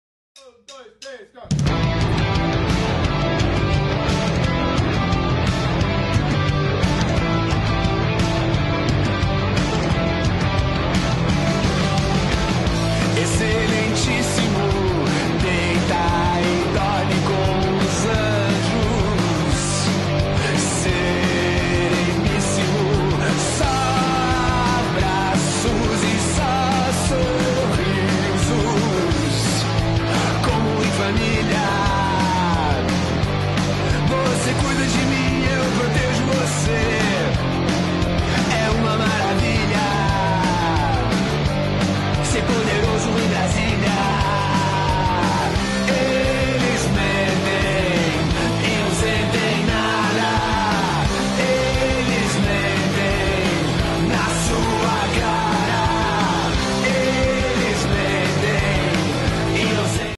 Fora Renan em Maceió faz projeção na casa do senador
O MBR (Movimento Brasil) e o Livres Alagoas protagonizaram um show de protesto na praça do Alagoinhas, em frente ao Edifício Tartana, residência do senador Renan Calheiros em Maceió - AL, para dizermos NÃO à sua candidatura para a presidência do Senado.